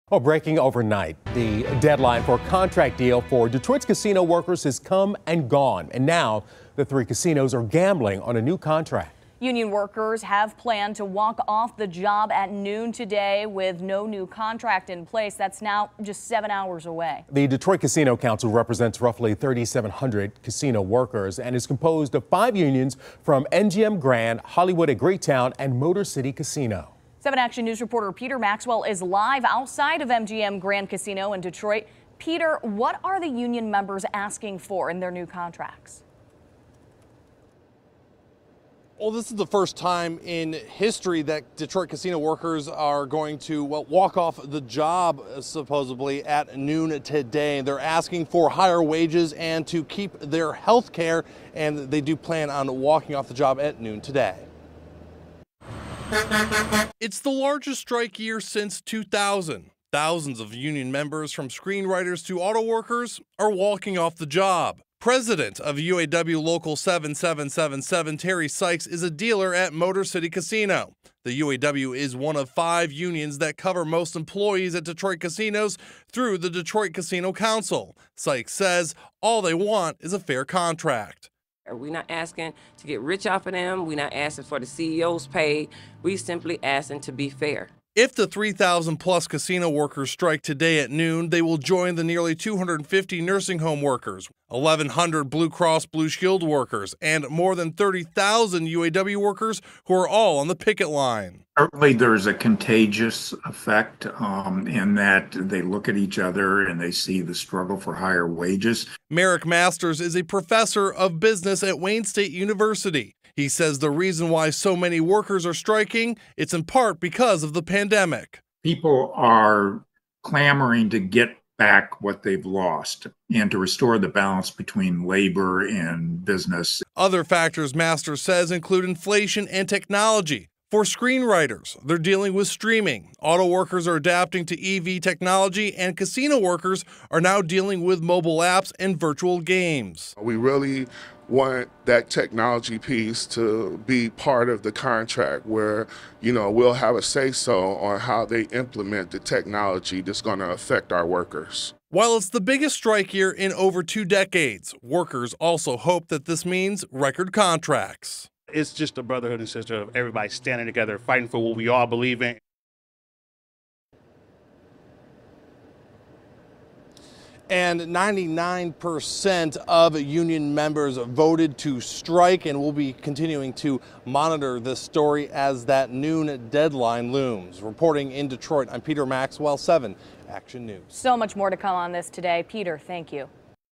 Detroit casino workers could begin striking by Tuesday afternoon
It's been called the "year of the strike." From screen writers to auto workers, unions are walking off the job. Soon, workers at Detroit casinos could join them.  “We’re not asking to get rich off of them, we’re not asking for the CEO’s pay, we’re just asking to be fair,” said Motor City Casino employee Terri Sykes, who is also president of UAW Local 7777.